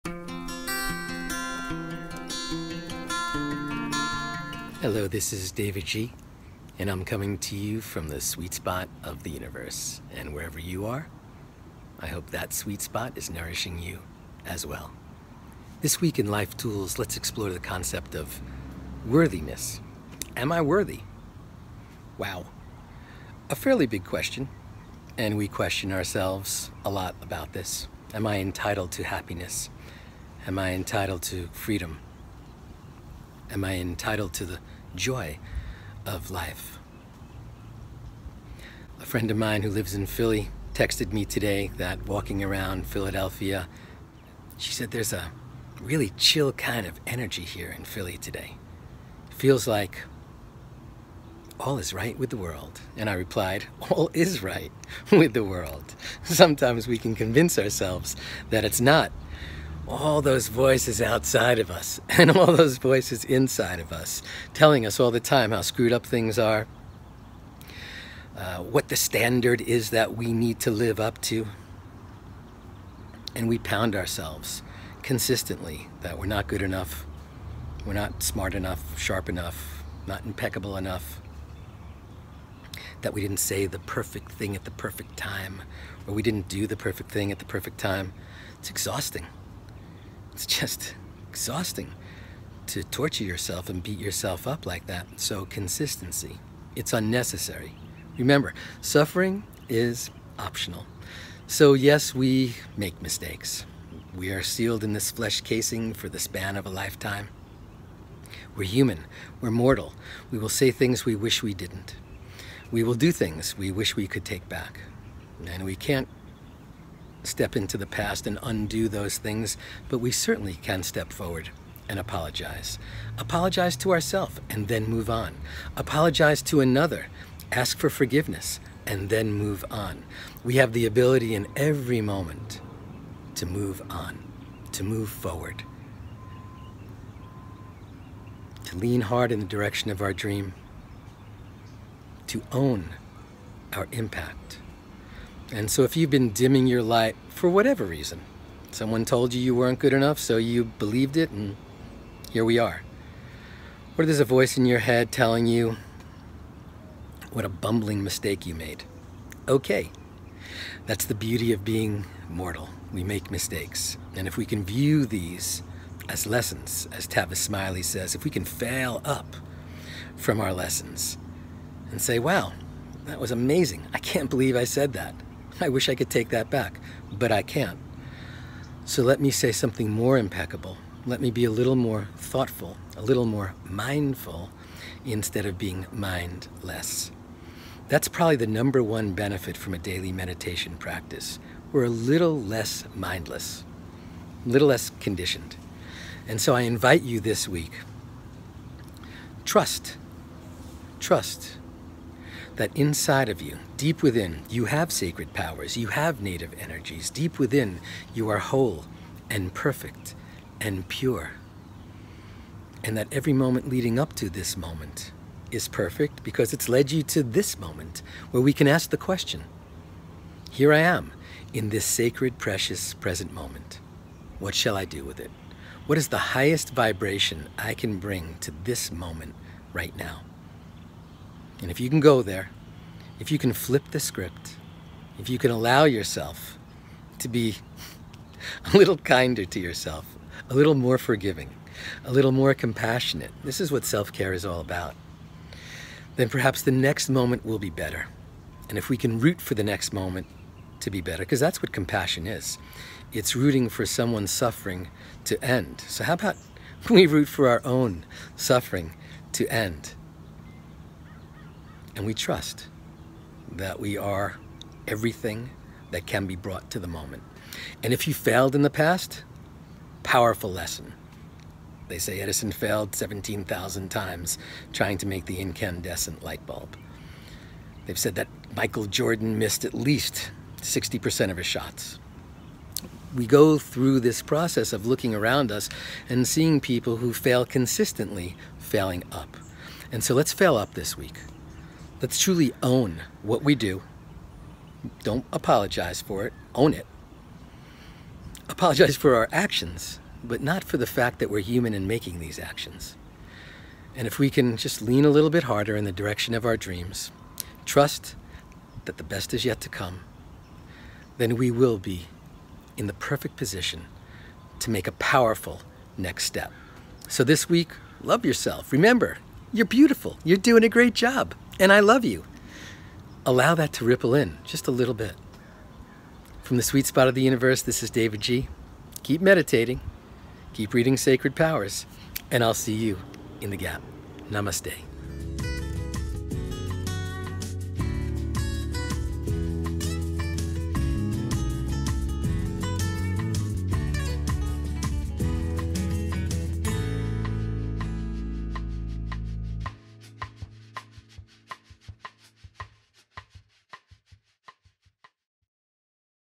Lean In & Move Forward | davidji
Believe it or not… suffering is optional. When it comes to the topic of worthiness you are entitled to happiness, freedom, and so much more. Worthiness is a big question that many of us ask ourselves regularly. When we hold ourselves to an unreasonable standard, it becomes more and more likely that we will convince ourselves that we aren’t good enough. At the end of the day, we are all human and we are all mortal. We shouldn’t hold ourselves to a standard of always doing the perfect thing or even saying the perfect thing at the perfect time. We can’t thrive all the time in every area of our life, but we can be gentle with ourselves to lean in and move forward.